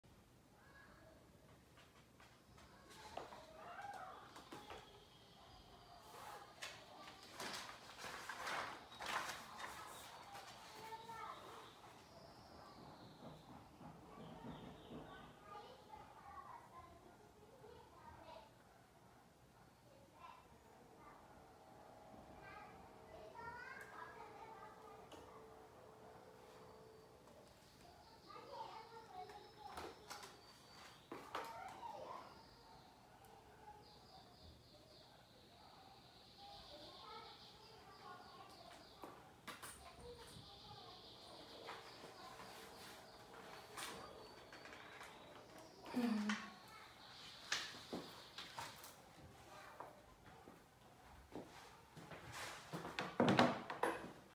Girl Burns Hair Off While Curling || ViralHog
Occurred on May 31, 2021 / Blumenau, Brazil

Hair falls out when doing curls.